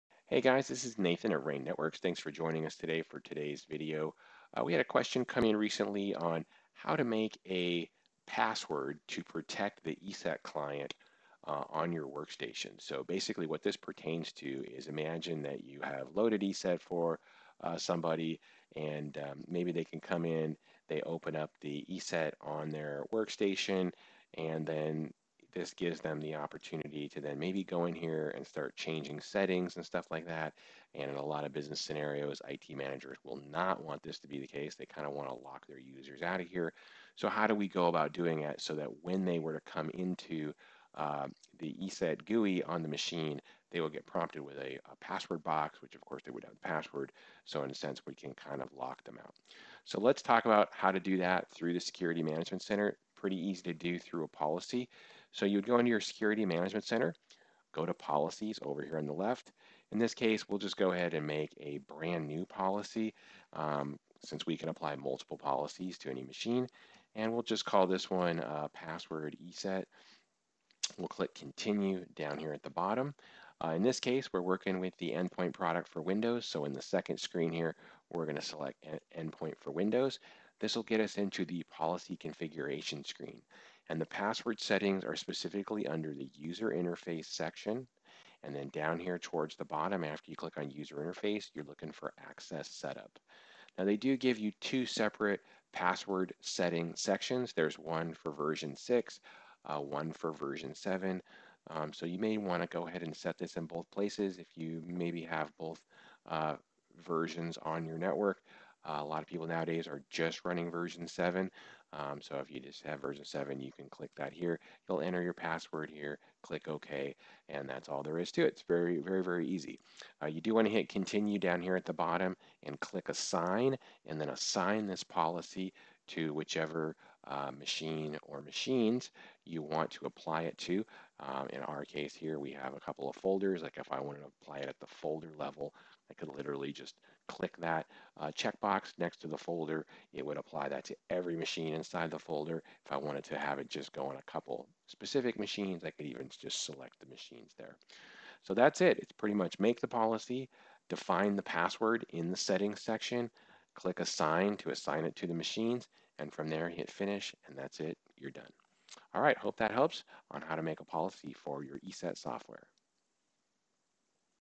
How to password protect your users in ESET Security Management Center!
Today Nathan from Rain Networks walks you through how password protect your users in ESET Security Management Center! This allows IT admins to make sure only authorized users are able to make changes to their computer’s security policies and ESET settings by requiring a password to access the ESET GUI on a machine.

If you have any questions about ESET, or anything IT related, drop a comment and we’ll get back to you ASAP!